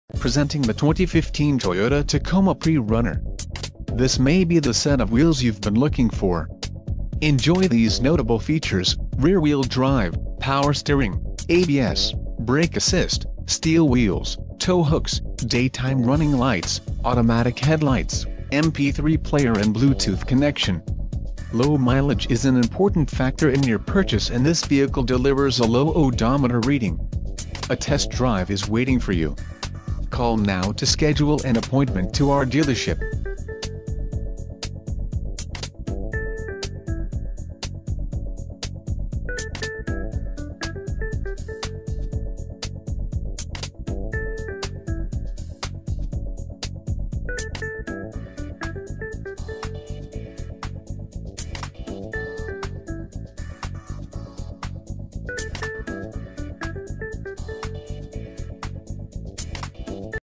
2015 Toyota Tacoma PreRunner - Stadium Toyota - Tampa, FL...
Built for action, our 2015 Toyota Tacoma Double Cab V6 in Super White has brains and brawn. Motivated by a 4.0 Liter engine producing 236hp on demand and paired with a 5 Speed automatic transmission, it attains near 20mpg on the open road. The perfect blend of size and capability, this sure-footed 4WD truck can handle your every adventure.    Our Double Cab V6 has a reputation for looking good and working hard. With a bold grille and aggressive stance, this athletic truck means business. You'll appreciate the convenience of hauling bikes, furniture or anything else with the bed utility rail system. Once inside the spacious interior, you'll notice upgraded upholstery on front bucket seats, a 60/40 split rear bench and plenty of leg room for rear seat passengers.